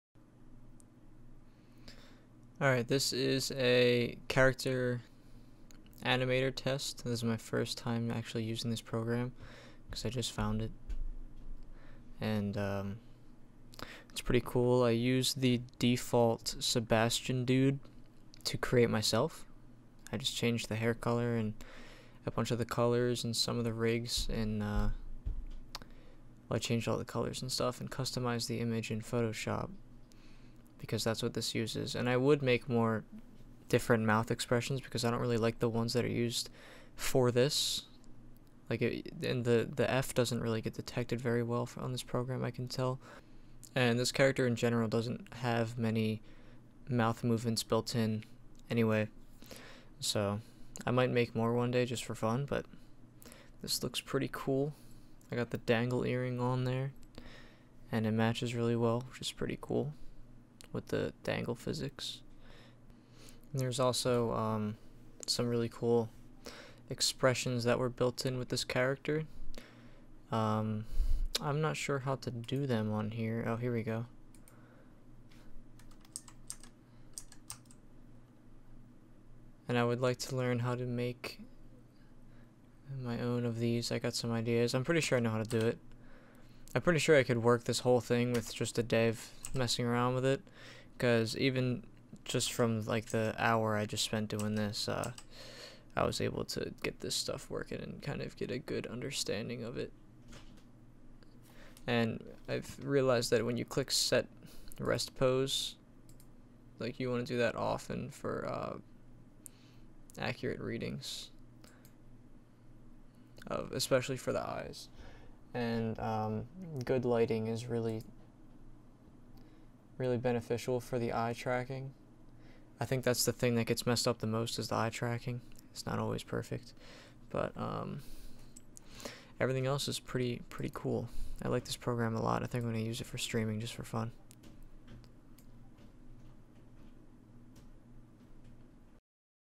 Adobe Character Animator Test- ANIME TOZO!
This was my first puppet! I really like this program and I just downloaded a bunch of new stuff to have fun with. So excited! Let me know what you think!